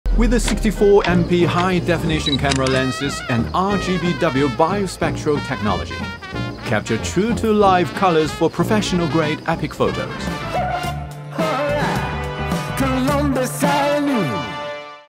TECNO PHANTOM V Flip 5G | 64MP Main Camera
It's so easy to capture true-to-life colors for professional-grade photos with PHANTOMVFlip5G which comes with 64MP Ultra-clear Main Camera.